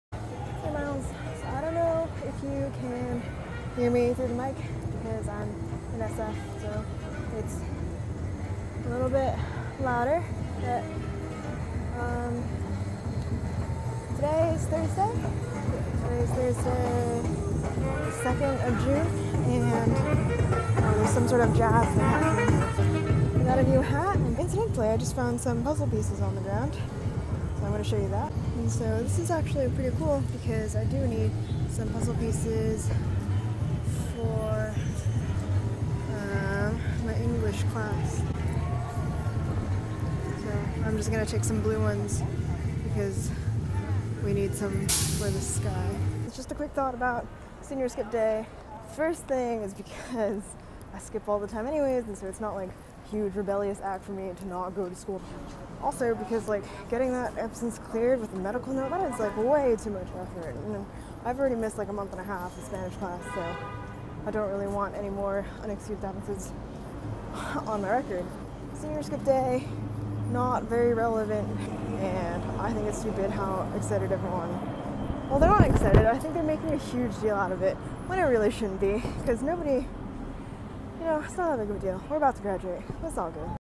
Puzzle Pieces and Senior Skip Day
In which Amy goes to SF, finds some puzzle pieces, rocks her new hat, and slams on Senior Skip Day.